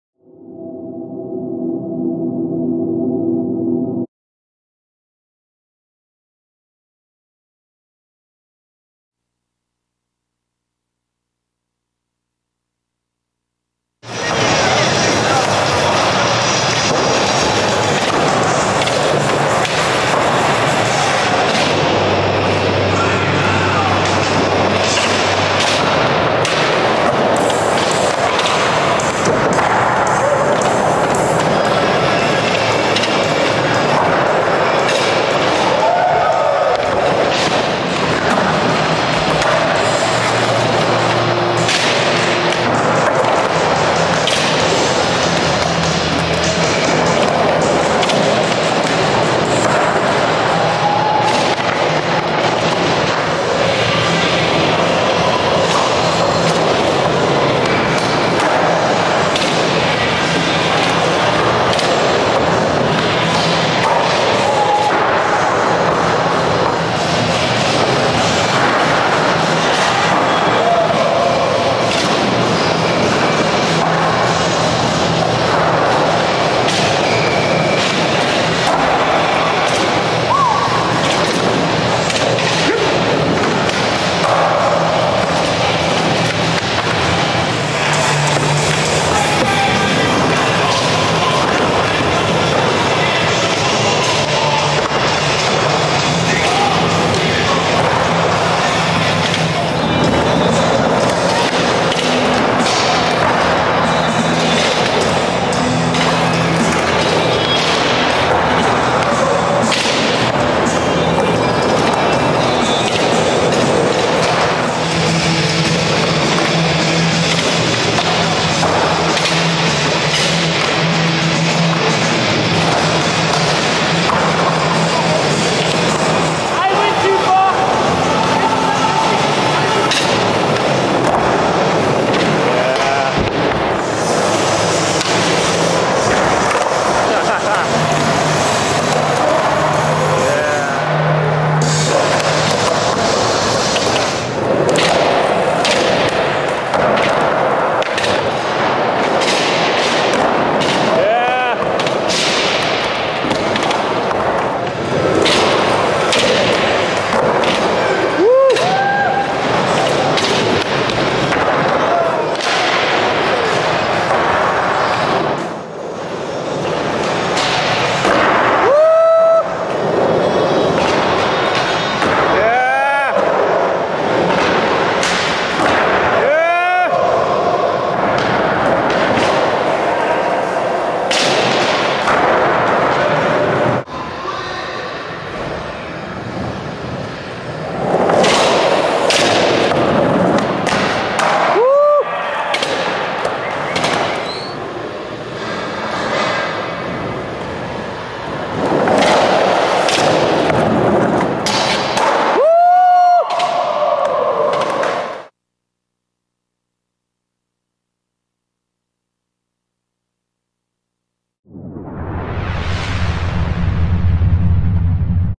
Dan MacFarlane 2003 skateboarding at Vans Skatepark of Houston
Session filmed the night before a big Mentality demo at Vans Skatepark of Houston. Mentality was thought of and began at this park! Thank you Steve VanDoren, all of the Vans employees, and locals!